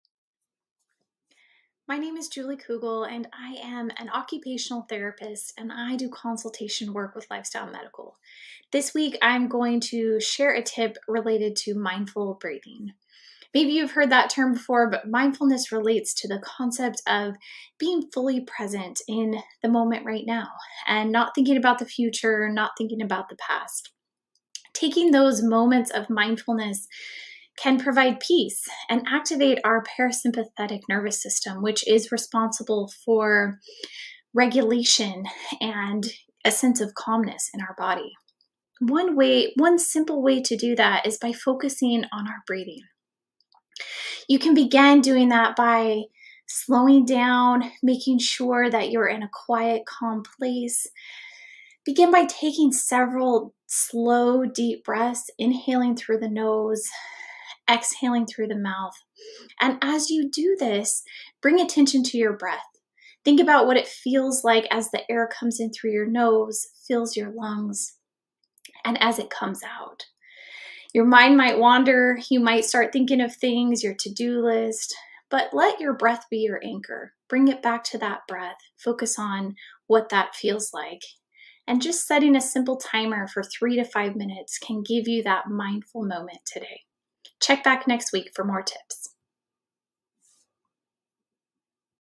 Lifestyle Tips for Longevity: Julie Kugel, OTD, shares tips on mindful breathing
Slow down, reconnect, and savor the present moment through this simple practice of mindful breathing.